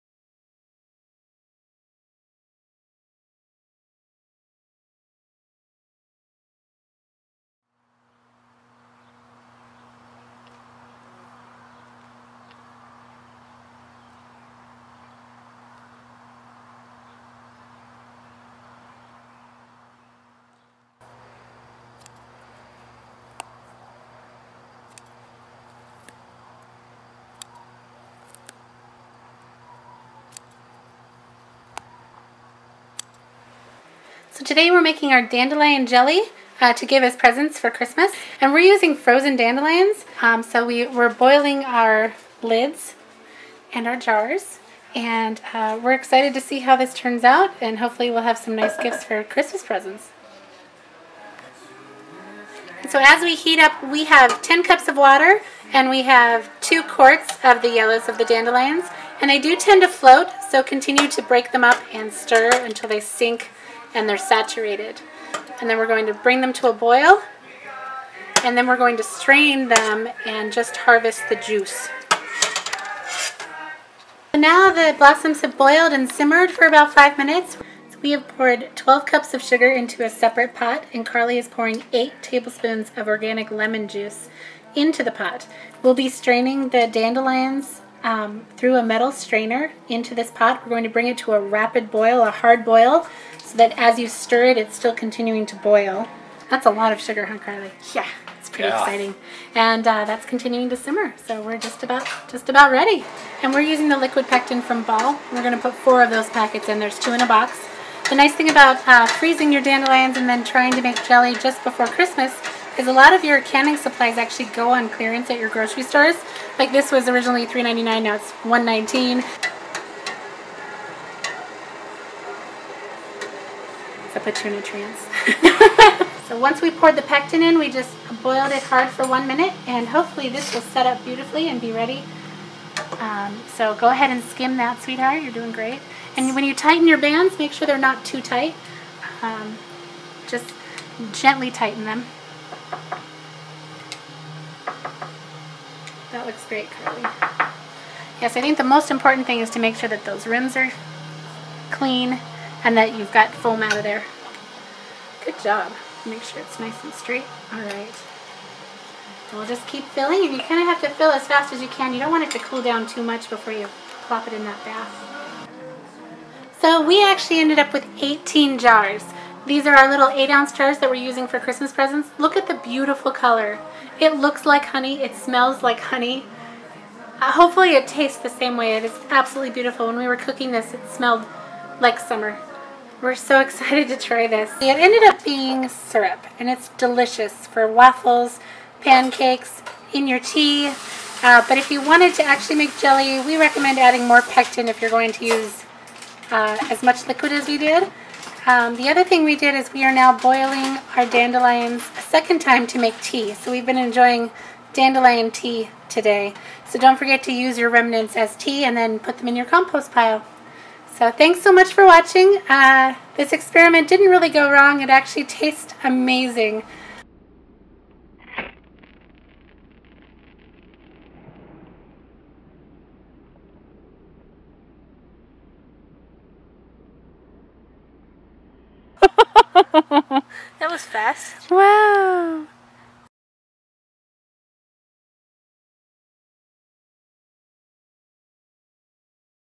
Easy Dandelion Syrup and Jelly, Cooking 101. Perfect for Holiday Gifts!
Our adventure making Dandelion Jelly. We recommend using four to six packets liquid pectin for syrup OR eight for jelly...or maybe try less liquid, or powdered pectin instead?  taste is AMAZING, but you may find your own experiments adjusting pectin and liquid work better. Let us know how yours turned out? You are rays of sunshine lighting our way, we appreciate you all! Have a beautiful day!